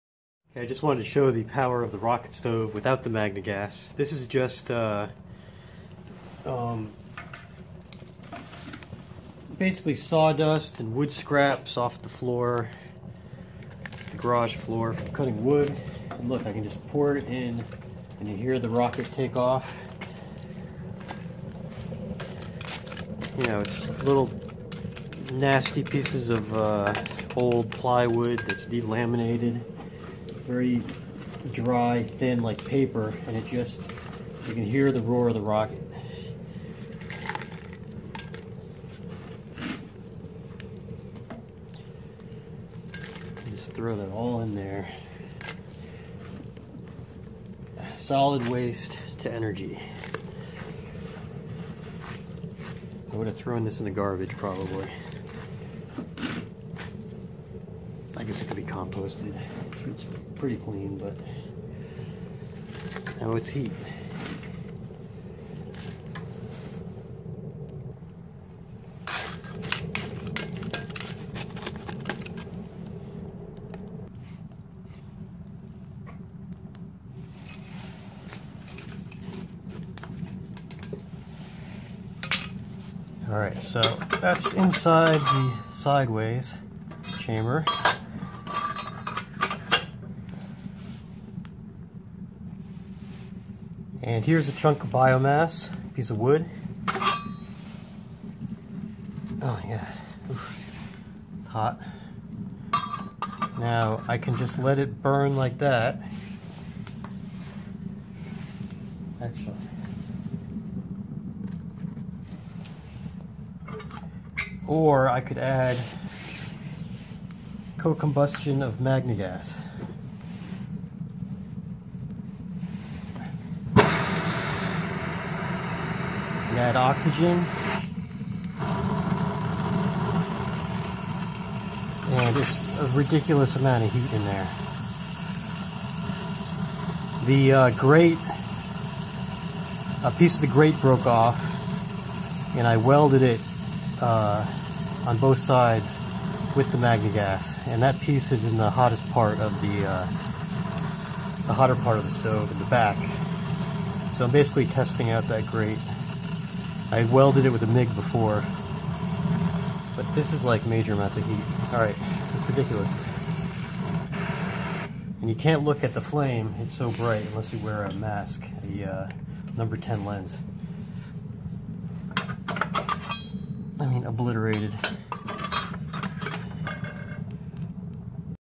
ManCave Rocket Stove Heater ScrapDust Fuel & MG2
Showing off the amount of heat I can get from just sweeping the floor of wood scraps & sawdust.  I made this rocket stove with the help of Kevin Bacon, I didn't have a wide piece of square pipe for the top loader so I used what I had which is a bit smaller and sometimes I regret it when loading sticks but it seems to work well with the dust as air can rush in around the sides of the horizontal burn chamber...-my theory- I should try throwing pellets in there too and see how that works, I suspect I will need to make a pellet basket though, would be interesting to see the kind of heat and burn time I could get.
After I show this I show some Primary Co-Combustion using my MagneGas welding torch,  I throw a little piece of BioMass ie. wood chunk into the chamber and obliterate it with the 10,000 degree Fahrenheit oxy/MagneGas2 Flame.  Increased heat means increased efficiency and lower emissions.  That's the basis of the Rocket Stove which is why they use so much less fuel than regular wood stoves and have little smoke once hot and running.